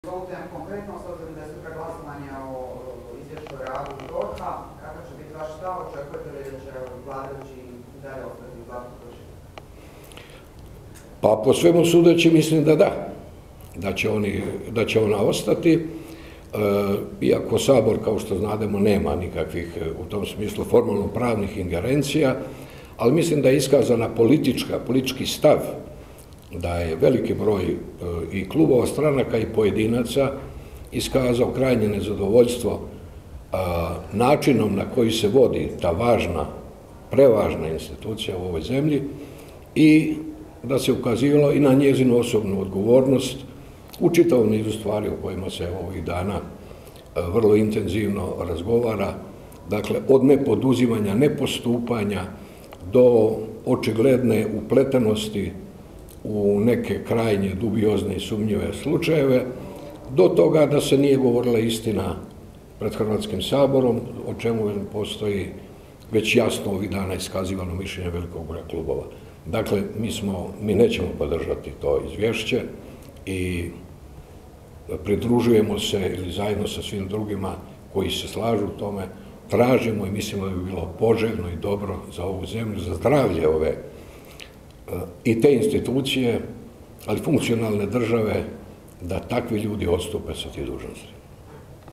Vidović: "Nećemo podržati isvješće državne odvjetnice Zlate Hrvoj Šipek"
Na konferenci za medije Kluba zastupnika Socijaldemokrati o temi „Prijeti li Hrvatskoj kolaps sustava socijalne skrbi“ govorili su zastupnici Davorko Vidović, Ivana Posavec Krivec i Irena Šimunić.